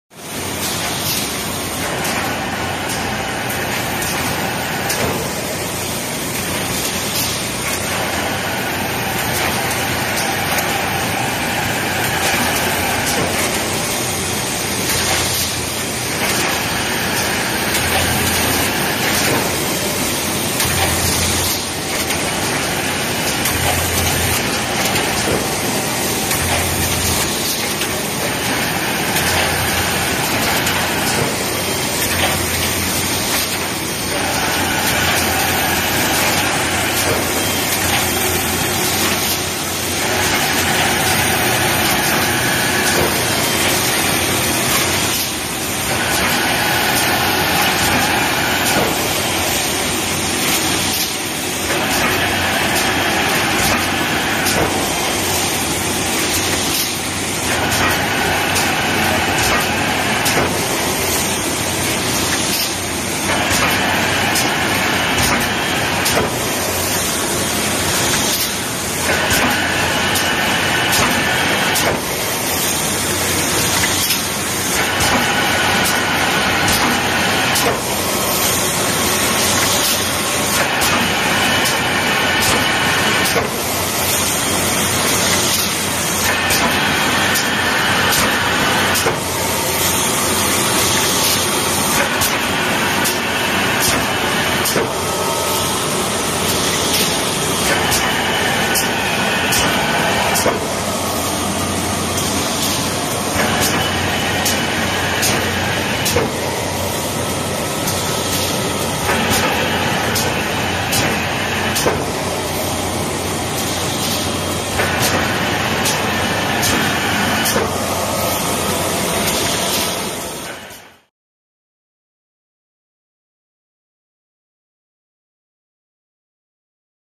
Fully Automatic Bag Top Hemming Conversion Line (Model no. Hemitec)
Fully Automatic Bag Top Hemming Conversion Line
(In-Line Process for Cutting – Sewing–Bag Top Hemming for Woven Bag)
Model No.: HEMITEC